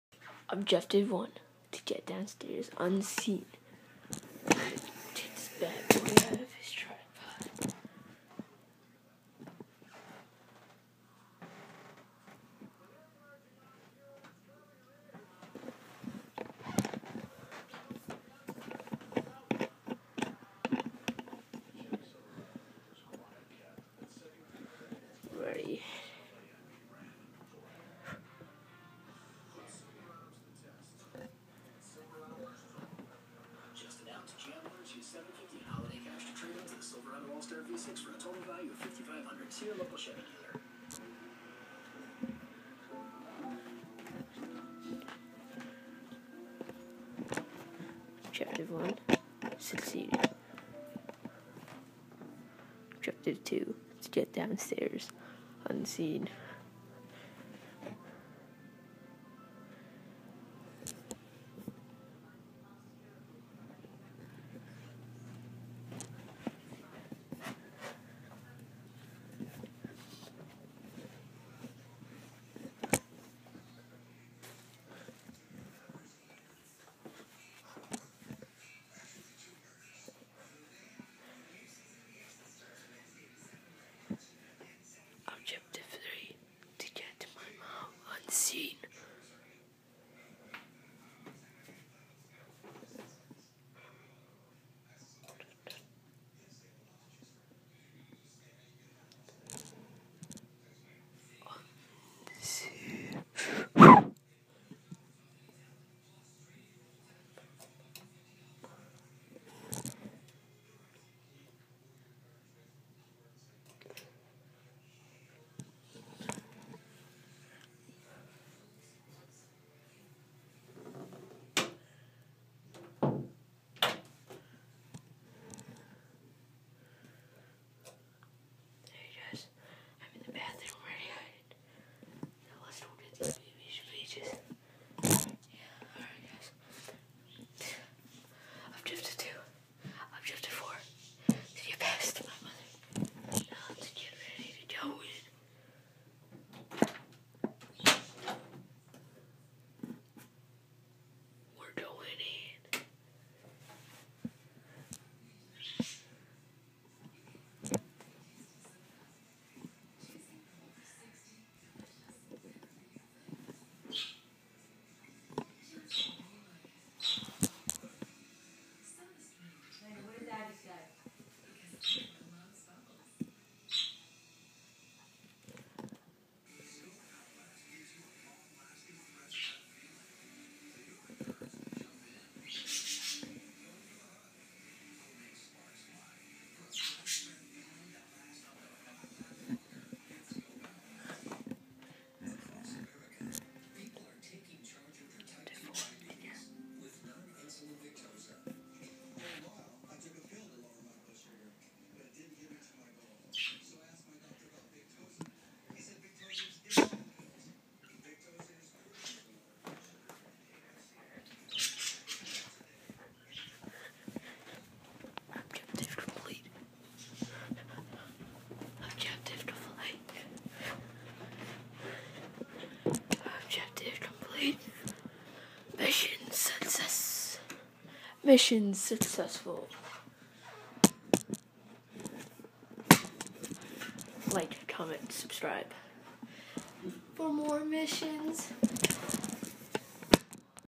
Mission 1: to get a pic of my mom
Ian gets a tripod then takes it on a and try to get a picture of his mom's mission one